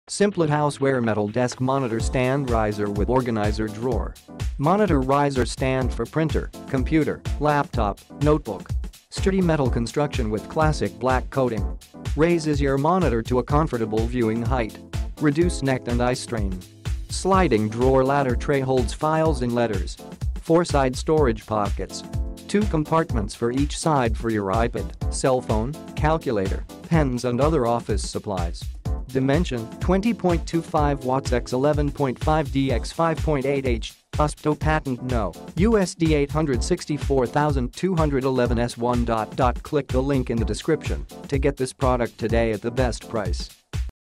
SimpleHouseware Metal Desk Monitor Stand Riser with Organizer Drawer Review
Monitor Riser / Stand for printer, computer, laptop, notebook.
Sturdy Metal Construction with Classic Black Coating
Raises your monitor to a confortable viewing height. Reduce neck and eye Strain
Sliding Drawer Latter Tray holds files and letters. 4 Side Storage Pockets. Two compartments for each side for your ipad, cell phone, calculator, pens and other office supplies.
Dimension: 20.25"W x 11.5"D x 5.8"H; USPTO Patent No: USD864211S1

hot deals product review